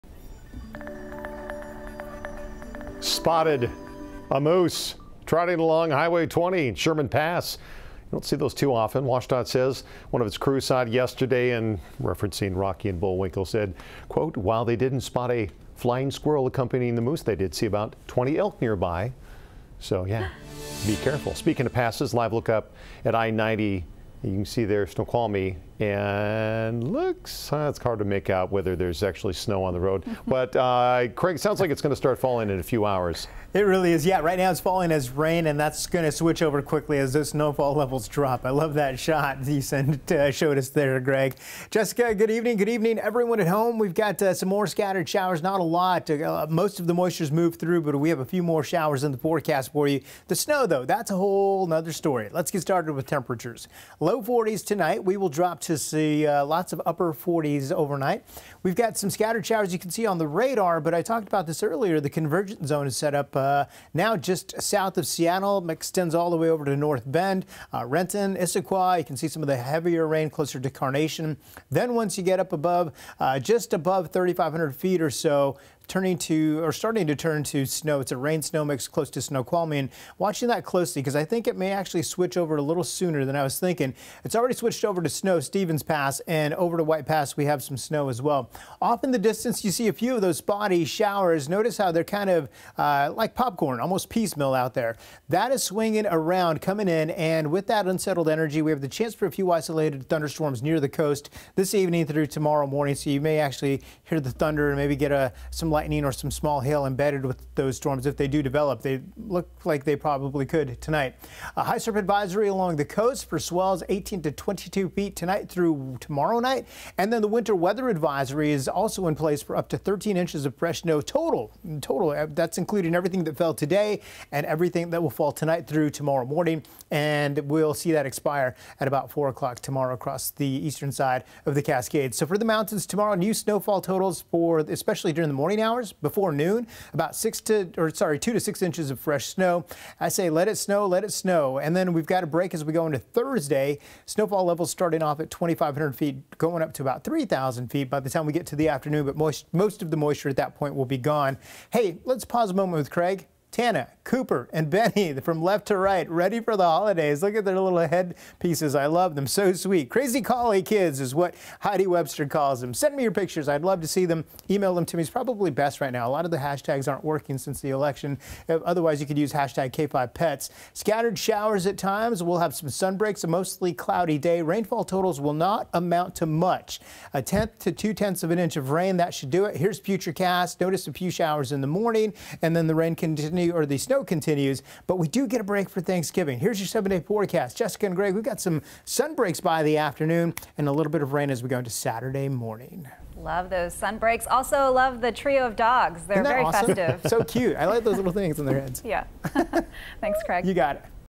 KING 5 Weather
KING 5 weather forecast with meteorologist Craig Herrera on Nov. 24, 2020, at 9 p.m.